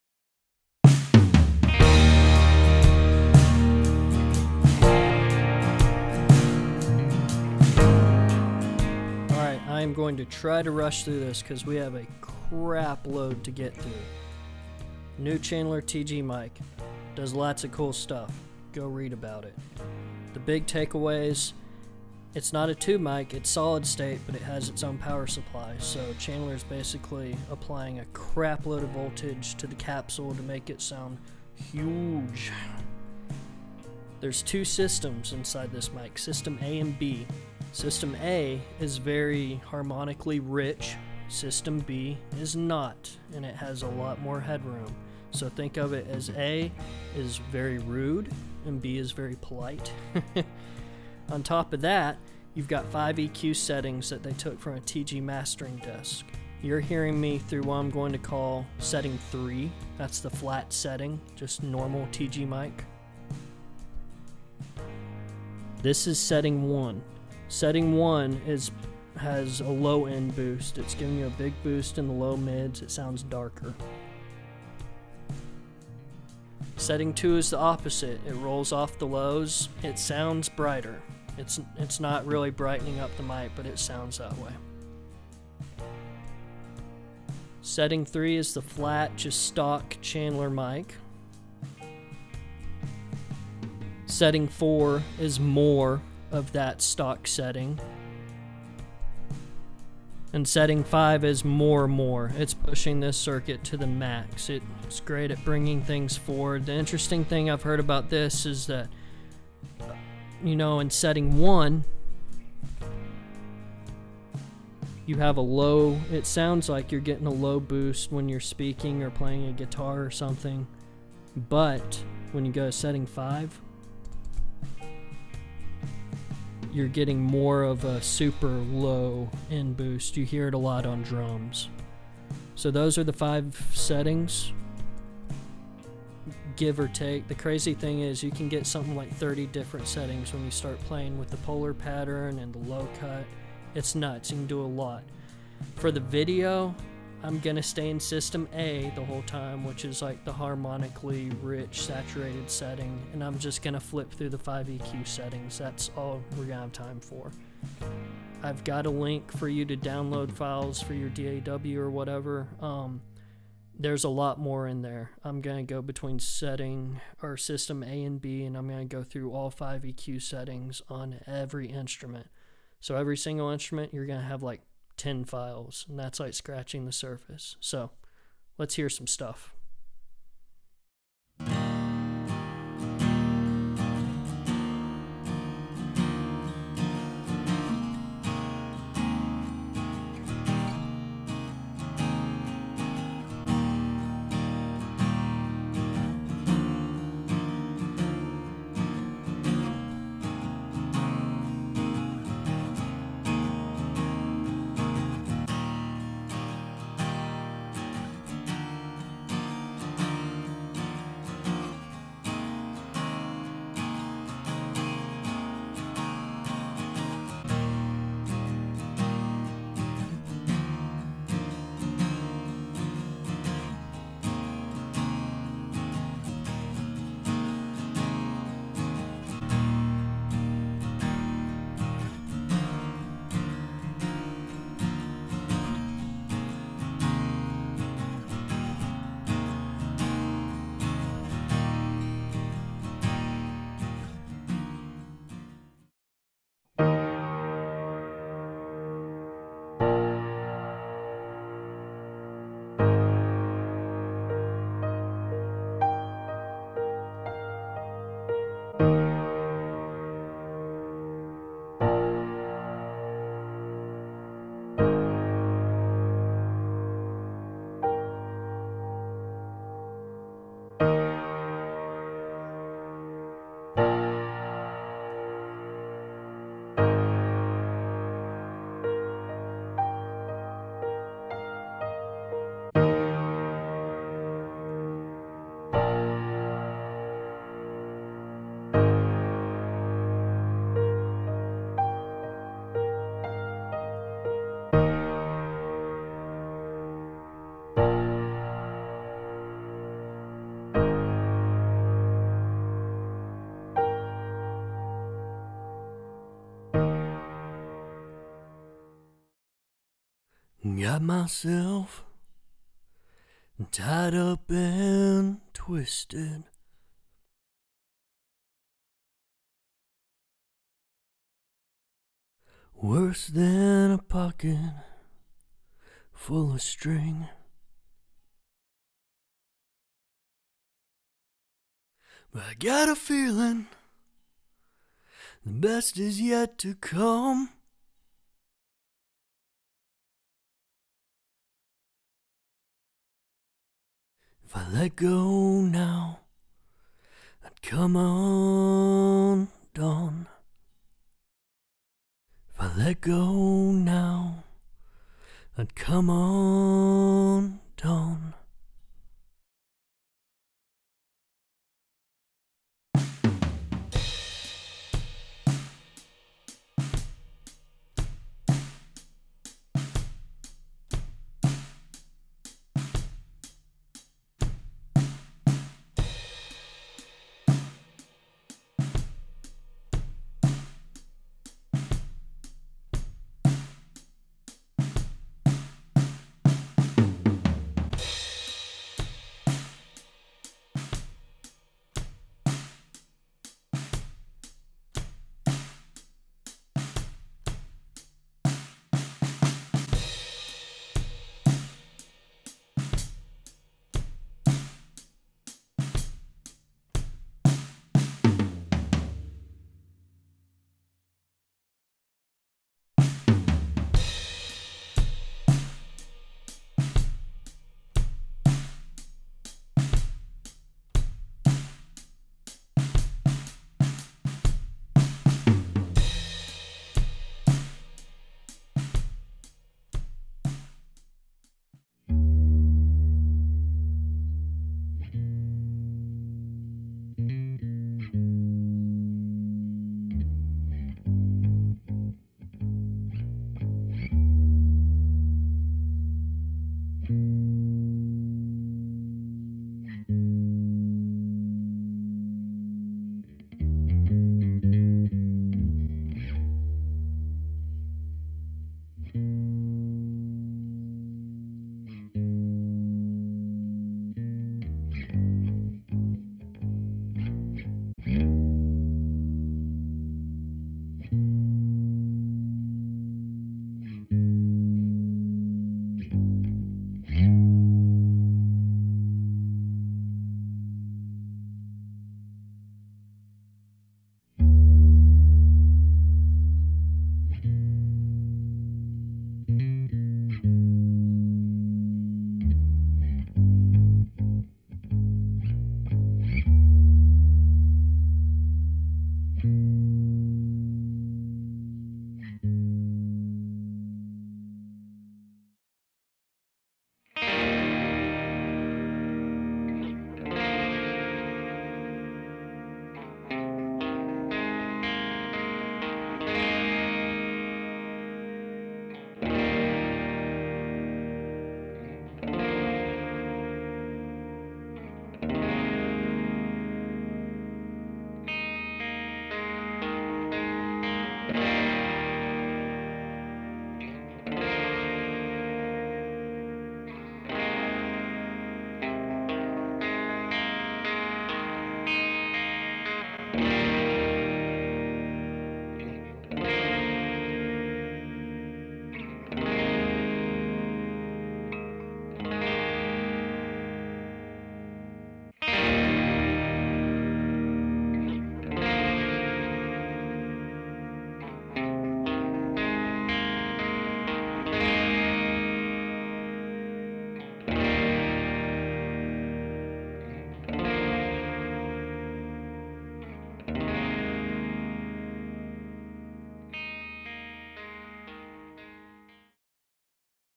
Chandler TG Microphone Review
We put the new Chandler TG Microphone through its paces and go through a lot the different settings. All recording was done with an AEA TRP preamp to impart minimal coloration to the signal.

If you use ProTools, all the settings are labeled and playlisted on each track. If you're not using ProTools, all files are labeled and located in the "audio" folder.